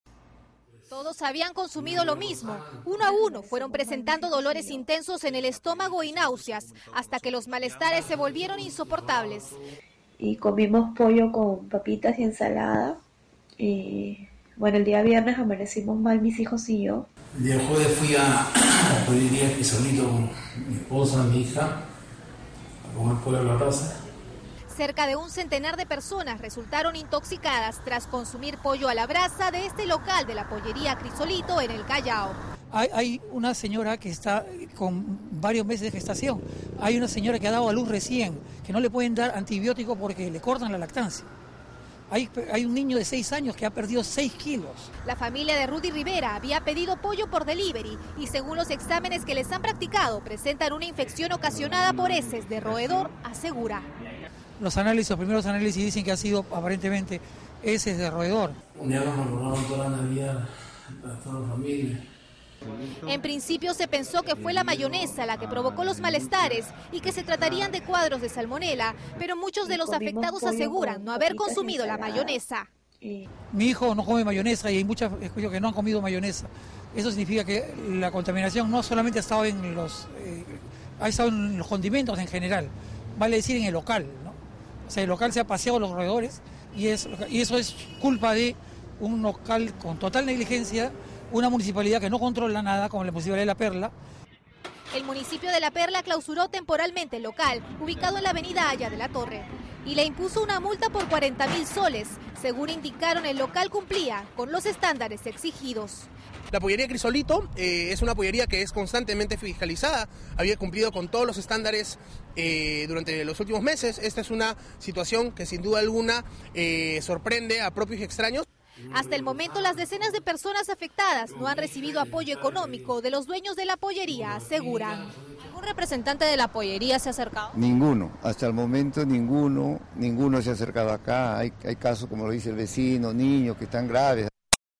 Casi un centenar de personas sufren intoxicación tras ingerir pollo a la brasa
Quienes consumieron el pollo fueron presentando dolores intensos en el estómago y nauseas hasta que los malestares se volvieron insoportables.